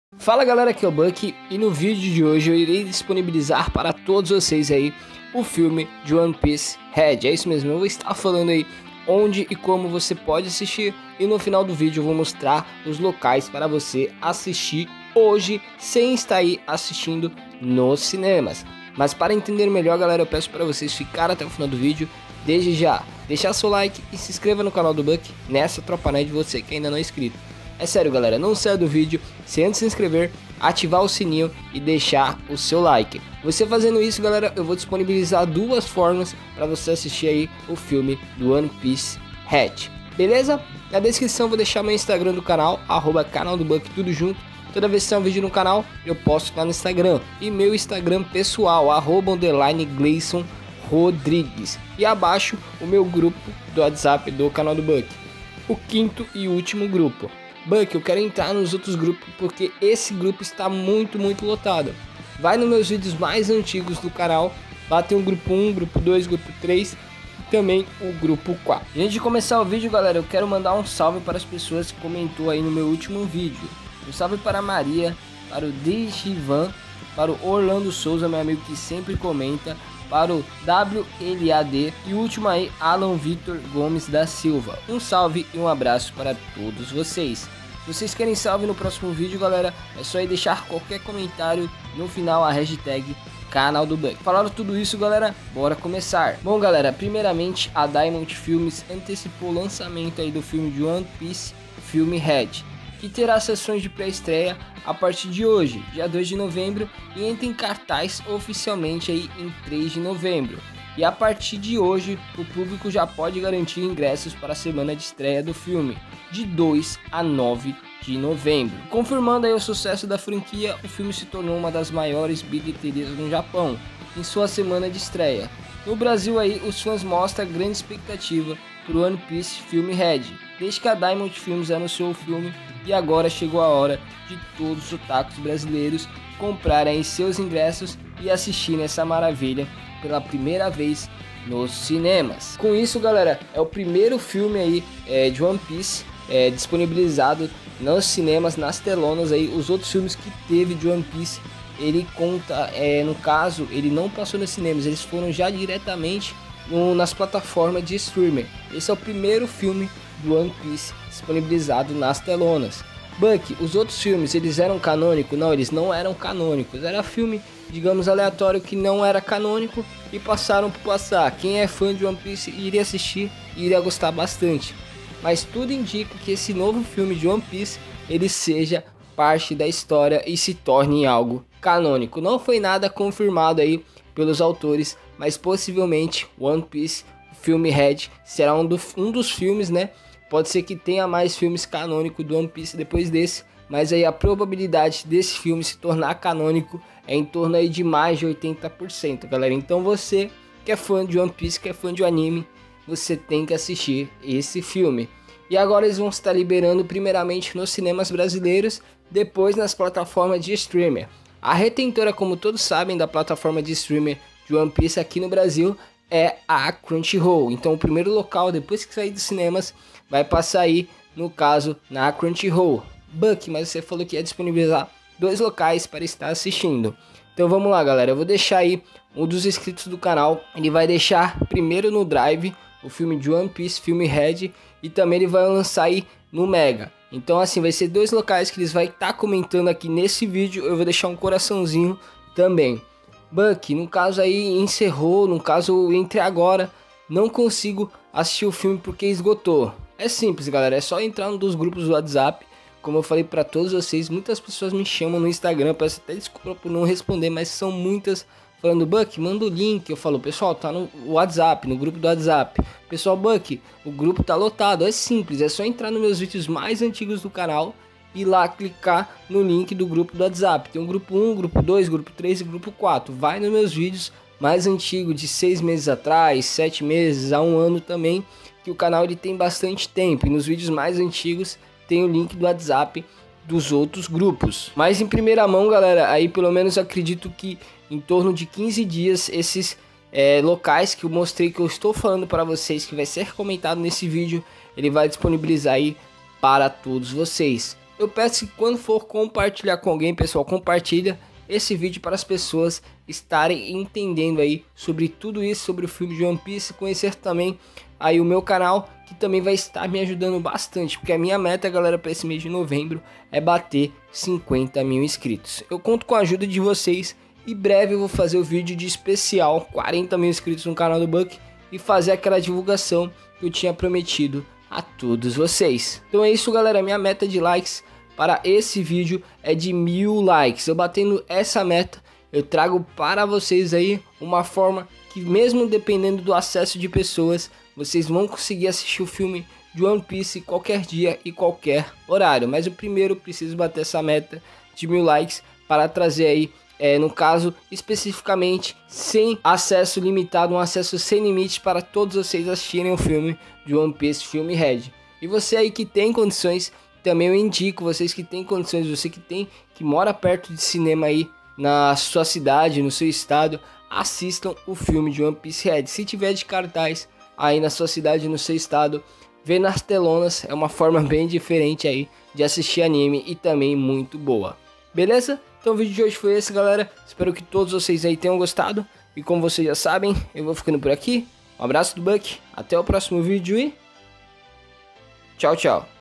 ONDE E COMO ASSISTIR?! ONE PIECE RED EM PORTUGUÊS PT/BR
Fala galera no vídeo de hoje eu vim falar para vocês onde e como assistir o filme de One Piece RED completo em português pt/br, é isso mesmo onde assistir o filme de One Piece RED em português! Vem com o Bucky❤️